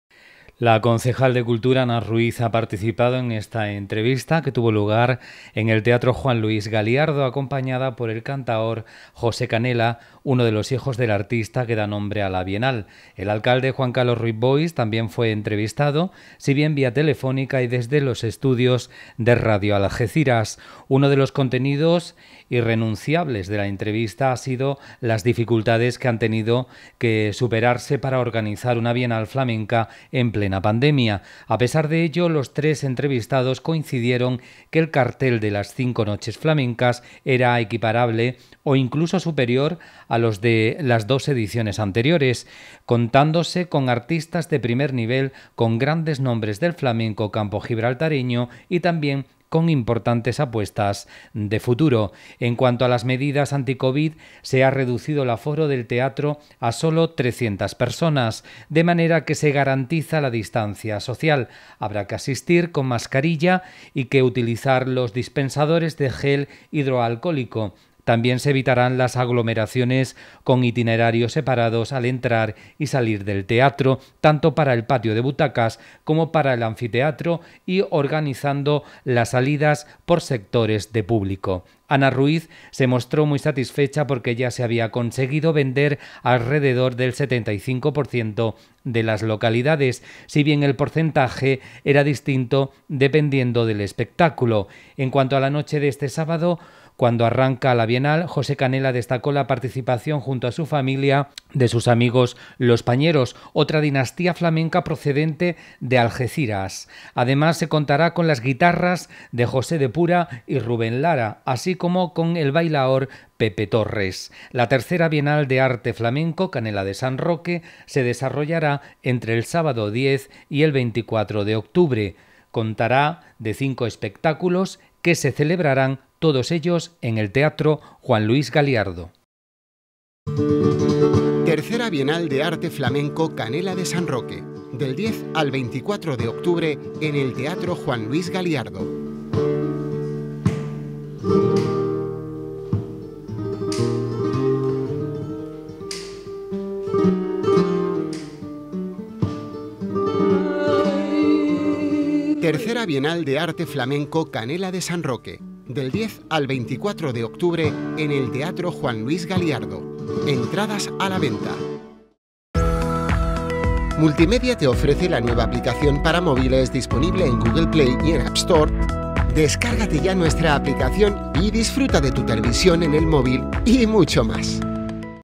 NOTICIA PROMOCION III BIENAL CANELA SAN ROQUE 7 OCTUBRE 2020
Intensa labor de promoción de la III Bienal “Canela de San Roque”, que comienza el sábado
La III Bienal de Arte Flamenco “Canela de San Roque” comienza a calentar motores de cara al espectáculo de arranque, titulado “Canelas y Pañeros”, que tendrá lugar este sábado 10 en el Teatro Juan Luis Galiardo. La organización está volcada en estos días en la labor de promoción, con entrevistas en distintos medios de comunicación de alcance comarcal y andaluz, como la realizada hoy miércoles por Radio Algeciras SER para el programa “Hoy por hoy Campo de Gibraltar”.